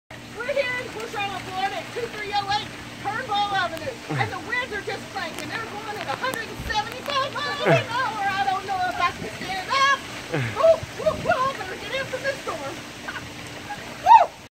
HURRICANE IRMA. 170mph wind live broadcast
Live broadcast for Hurricane Irma Port Charlotte Florida!!